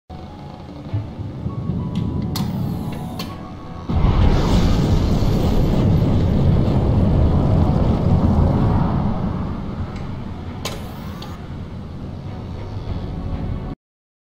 Aerodynamic Missiles Hatch
A new aero dynamic missile hatch for my missile hatches mod.